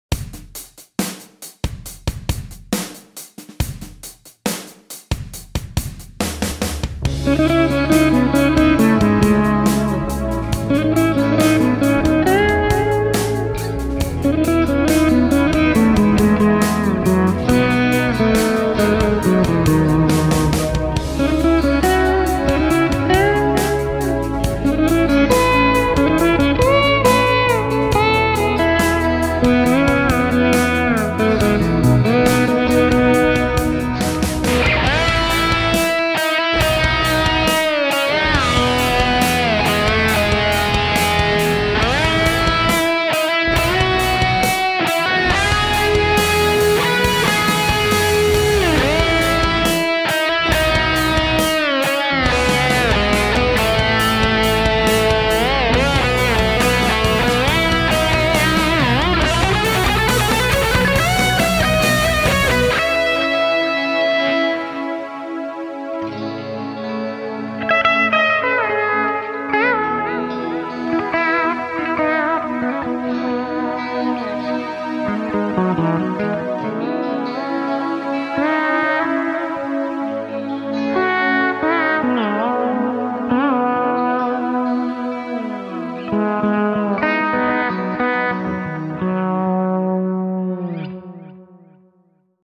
FRETLESS GUITAR - The awesome sound of my Vigier Excalibur Surfreter Model Electric Guitar
Here is a demo of my Vigier Excalibur Surfreter Model Fretless Guitar.

This particular clip was my entry for the 2020 "Jam Like A Boss Competition."